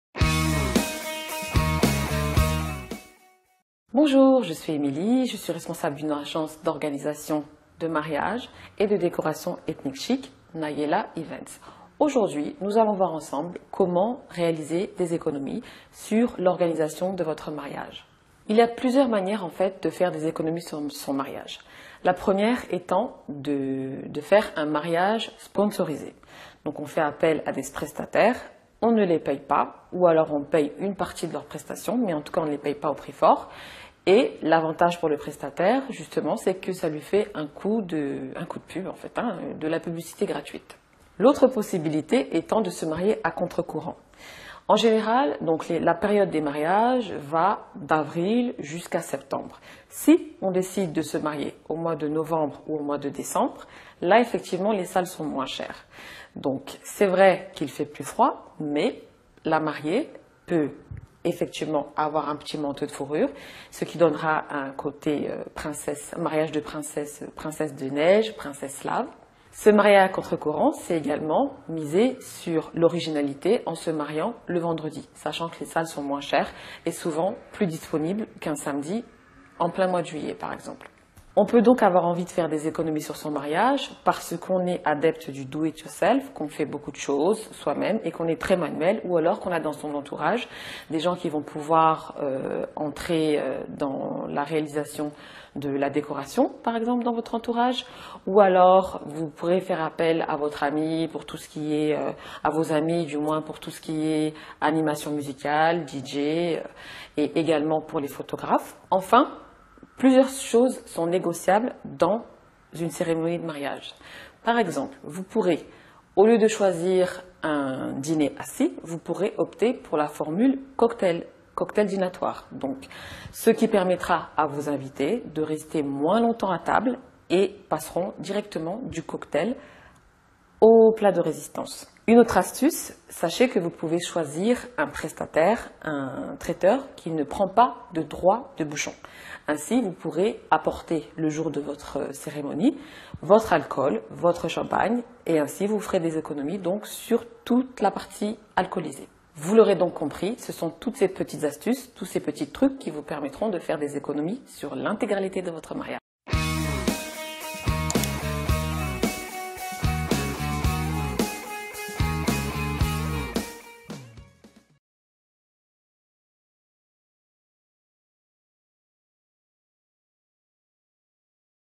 Mariage : faire des économies sur l'organisation
Comment faire des économies sur l'organisation d'un mariage ? Pour en savoir plus, notez bien les informations apportées dans cette vidéo. .

Émilie Mendy, responsable de l'agence Nayela Events (Nayelaevents.fr) vous explique comment réaliser des économies sur l'organisation de votre mariage.